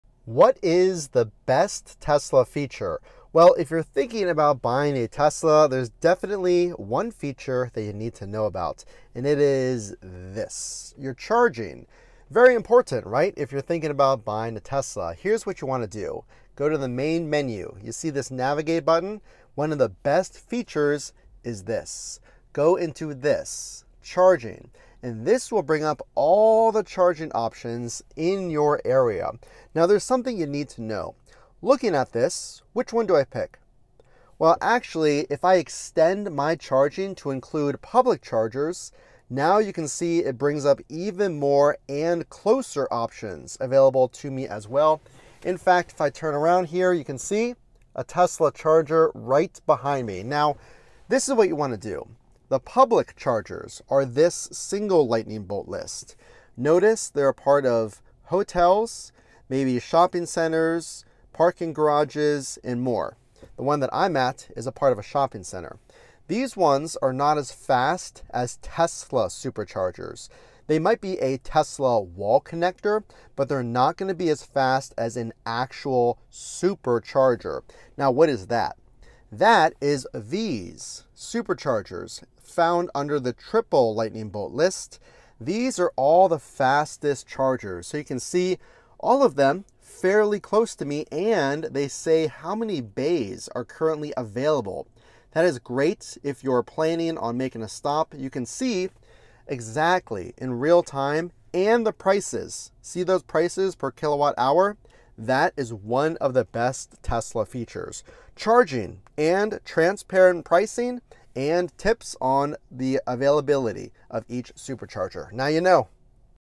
Tesla Secret 🤫 Charger Locator - MUST SEE! ⚡️
Discover Tesla's amazing charging feature! We show you how to find nearby chargers, compare speeds, and see real-time availability for stress-charging. Get transparent pricing tips now!